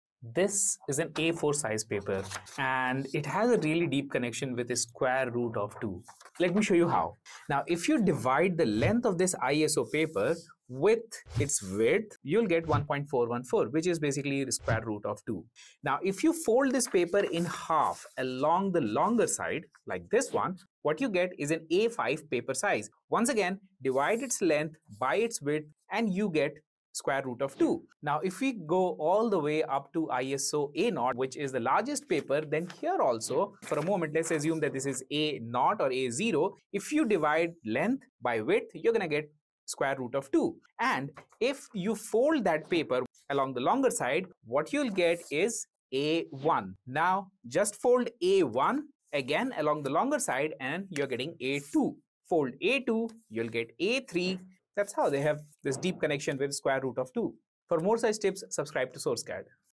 ISO paper size and squre root of 2
In this video, I have explained a deep connection between ISO paper sizes and the square root of 2.